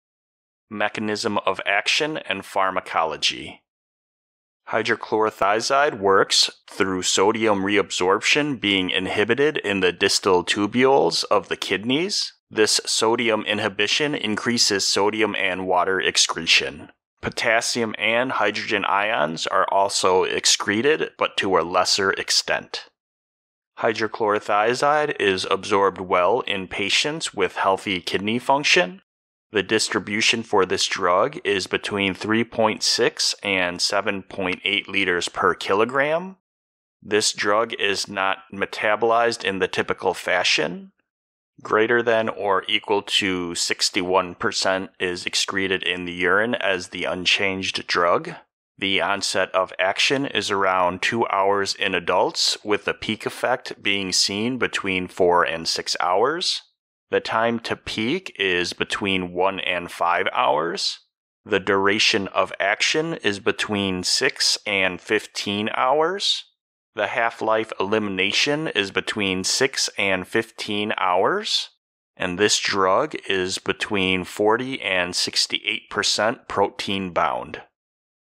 Hydrochlorothiazide Mechanism of Action & Pharmacology | Drug Cards Daily Clips
Clip taken from Drug Cards Daily Podcast Episode #14: hydrochlorothiazide (Microzide) | Edema, Hypertension, Calcium Nephrolithiasis Prevention, and Nephrogenic Diabetes Insipidus

Hydrochlorothiazide is a thiazide diuretic that goes by the brand name Microzide. The way this drug works is through the inhibition of sodium reabsorption in the distal tubules of the kidneys. The drug is most commonly used in the treatment of Edema but has other pharmacological uses. Typical edema treatment dosing is initiated at 25-100 mg po qd divided into 1-2 doses. Thiazide diuretics are not the preferred diuretic for edema but does have its place in therapy. There is a concern in patients with sulfonamide-derived drug allergies but this is often challenged.  The drug is excreted in the urine with ~60% as unchanged drug. Common side effects are orthostatic hypotension, diarrhea, dizziness, and headache. Substances such as alcohol may enhance orthostatic hypotension. Blood pressure, sodium, potassium and BUN should be monitored while patients are on this drug.

PASS YOUR BOARDS:
►RxPrep (Industry Leader)
►MPJE (Pharmacy Law)
►Flash Cards (Pharmacology)